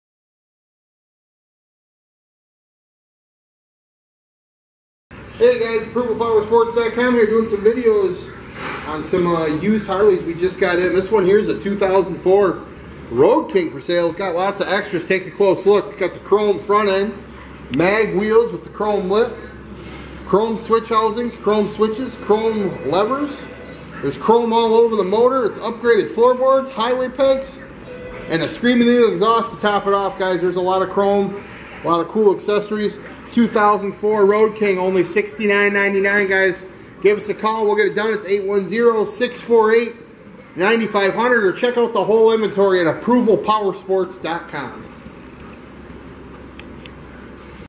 2004 HARLEY ROAD KING FLHRS U5476
USED HARLEY DAVIDSON ROAD KING FOR SALE WITH ALL THE EXTRAS! TAKE A CLOSE LOOK AT THIS ONE! CHROME FRONT END, MAG WHEELS WITH CHROME LIP, CHROME SWITCH HOUSINGS, CHROME SWITCHES, CHROME LEVERS, CHROME ALL OVER THE MOTOR, UPGRADED FLOOR BOARD, HIGHWAY PEGS AND THE SCREAMING EAGLE EXHAUST SOUNDS GREAT! THIS ONE IS A MUST SEE!